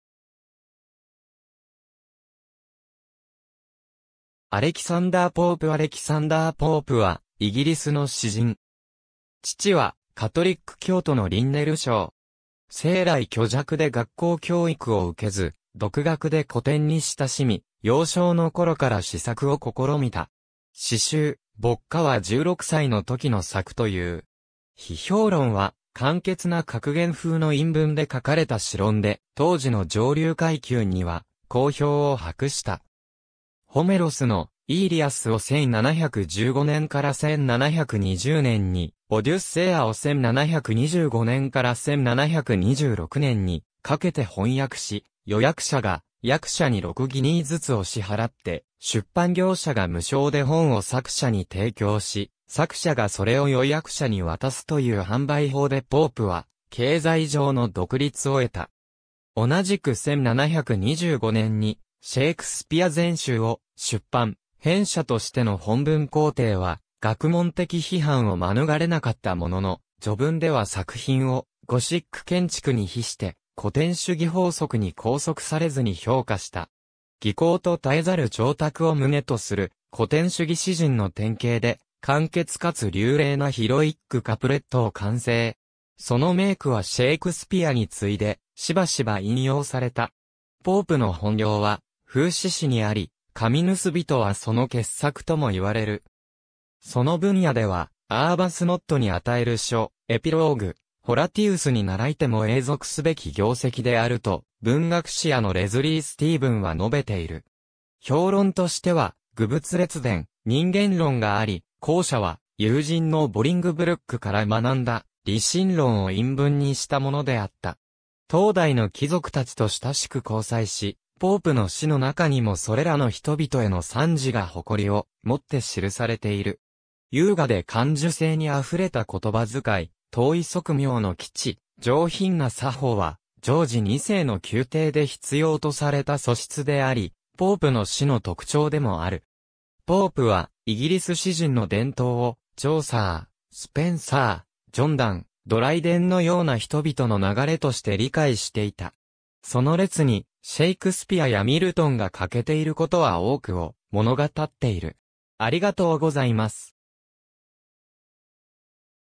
アレキサンダー・ポープ
シティ・オブ・ロンドン出身の人物
アレキサンダー・ポープ アレキサンダー・ポープ（Alexander Pope,1688年5月21日 - 1744年5月30日）はイギリスの詩人。
父はカトリック教徒のリンネル商。
生来虚弱で学校教育を受けず、独学で古典に親しみ、幼少の頃から詩作を試みた。
詩集『牧歌』（Pastorals, 1709年）は16歳の時の作という。
『批評論』（An essay on criticism, 1711年）は簡潔な格言風の韻文で書かれた詩論で、当時の上流階級には好評を博した。
ホメロスの『イーリアス』を1715年から1720年に、『オデュッセイア』を1725年から1726年にかけて翻訳し、予約者が訳者に6ギニーずつを支払って、出版業者が無償で本を作者に提供し、作者がそれを予約者に渡すという販売法でポープは経済上の独立を得た。
同じく1725年に『シェイクスピア全集』を出版、編者としての本文校訂は学問的批判を免れなかったものの、序文では作品をゴシック建築に比して古典主義法則に拘束されずに評価した。
技巧と絶えざる彫琢を旨とする古典主義詩人の典型で、簡潔かつ流麗なヒロイック・カプレット（heroic couplet, 弱強五歩格二行聯句）を完成。
その名句はシェイクスピアに次いでしばしば引用された。
ポープの本領は諷刺詩（Satire）にあり、『髪盗人』はその傑作ともいわれる。
その分野では『アーバスノットに与える書』『エピローグ』『ホラティウスに倣いて』も永続すべき業績であると文学史家のレズリー・スティーヴンは述べている。
評論としては『愚物列伝』（The Dunciad, 1728年 - 1742年）、『人間論』（An essay on man, 1732年 - 1734年）があり、後者は友人のボリングブルック（en）から学んだ理神論を韻文にしたものであった。
当代の貴族達と親しく交際し、ポープの詩の中にもそれらの人々への賛辞が誇りをもって記されている。
優雅で感受性にあふれた言葉遣い、当意即妙の機知、上品な作法はジョージ2世の宮廷で必要とされた素質であり、ポープの詩の特徴でもある。
ポープはイギリス詩人の伝統をチョーサー、スペンサー、ジョン・ダン、ドライデンのような人々の流れとして理解していた。
その列にシェークスピアやミルトンが欠けていることは多くを物語っている。